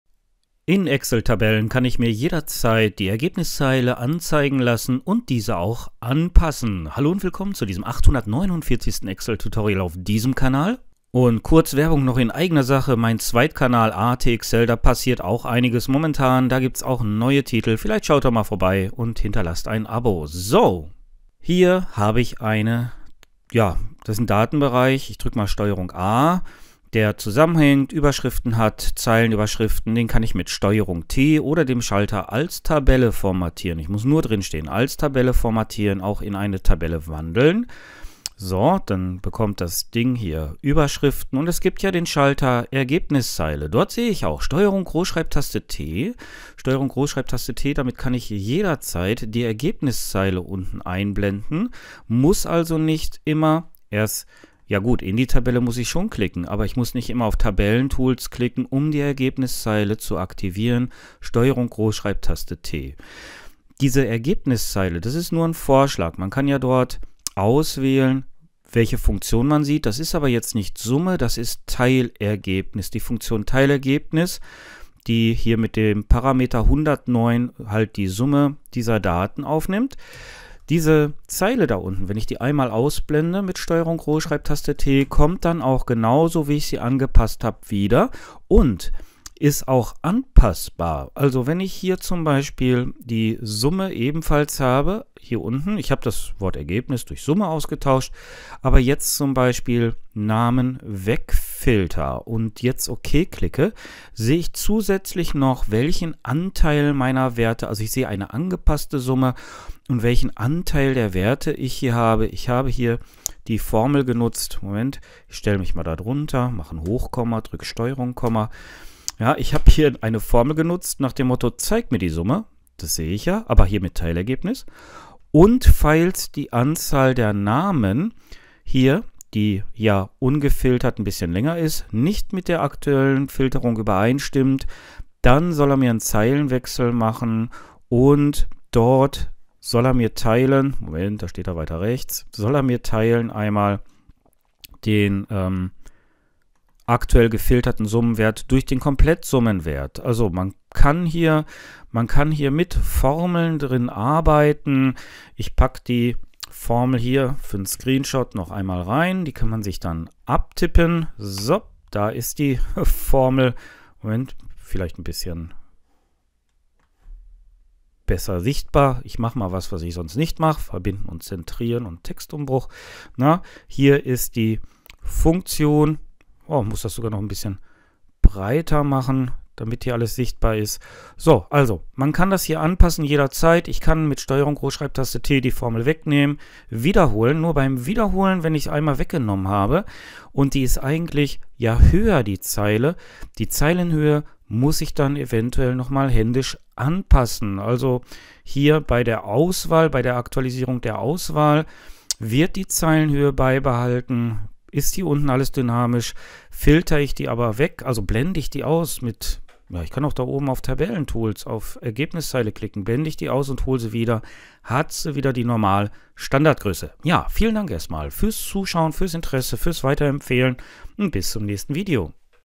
Ergebniszeile in Excel-Tabellen einblenden und anpassen
Die Ergebniszeile in Excel-Tabellen kann schnell per Tastenkombination angezeigt und auch angepasst werden. Mit eigenen Formeln kann dort ebenfalls gearbeitet werden.